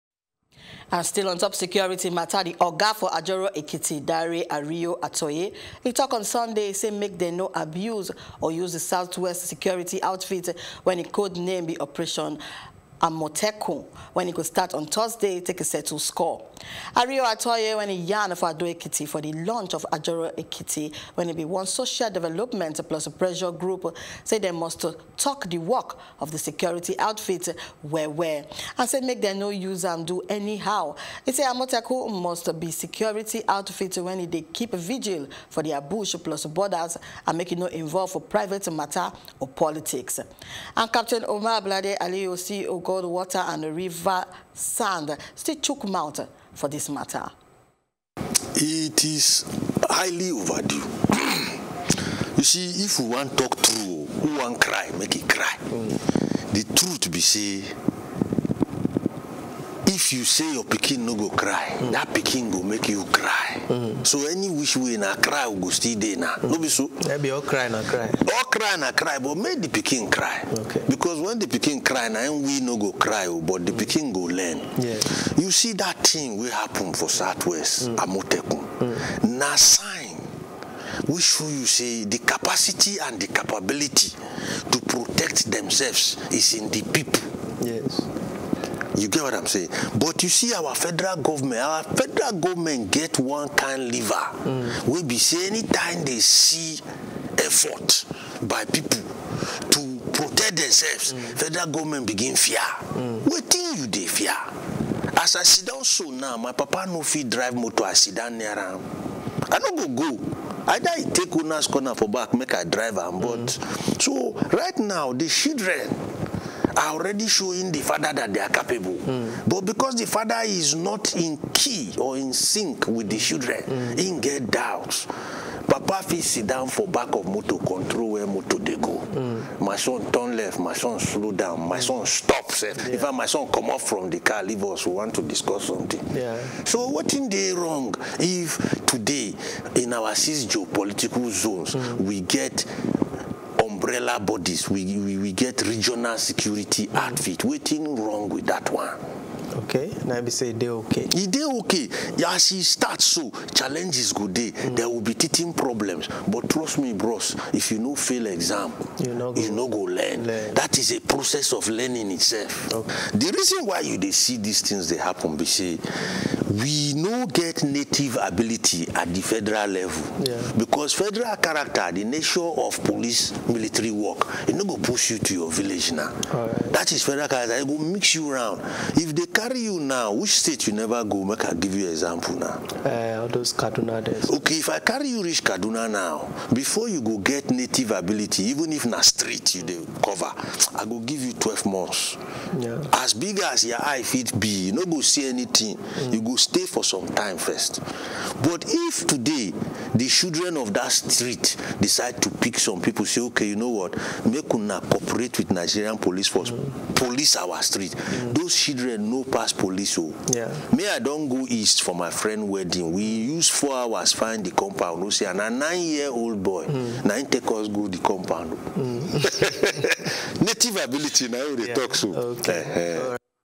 Group Say Make Dem No Use Security Initiative Settle Palava
The oga of Ajoro Ekiti, Dare Ariyo-Atoye, tok on Sunday say make dem nor abuse or use the Southwest security outfit, wen e  code-name be ‘Operation Amotekun’, wen e go start on Thursday take  settle score.

Captain Umar (Blade) Aliyu, CEO Goldwater & Riversand joined us in the studio.